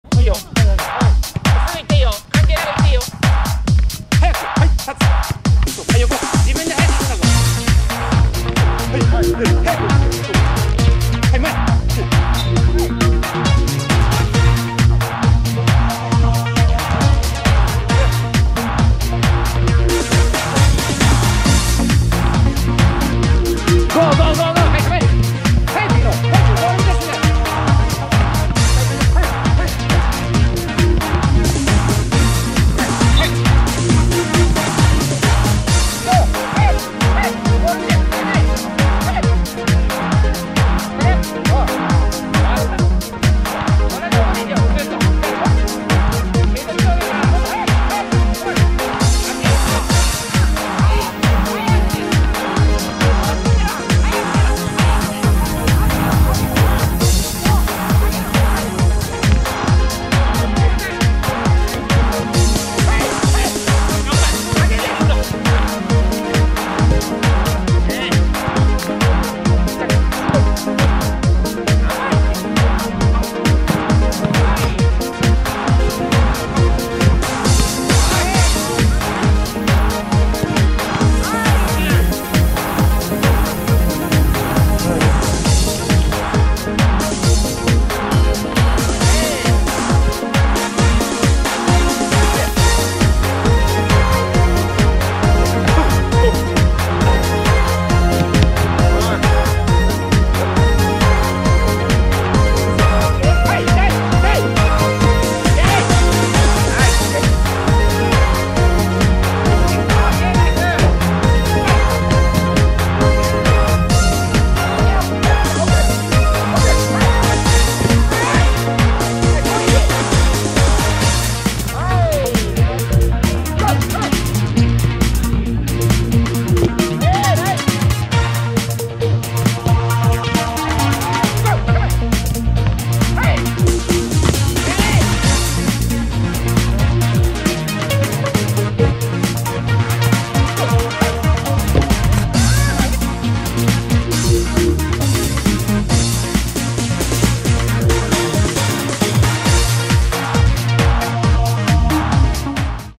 東福岡高校GKトレーニング！ゴールキーパーはスピードが命！
東福岡高校サッカー部のGKトレーニングを不定期で更新中！

Twitterではプレゼント企画なども実施中！

GKコーチはこちらのメルマガをどうぞ！

選手は福岡GKスクールへ！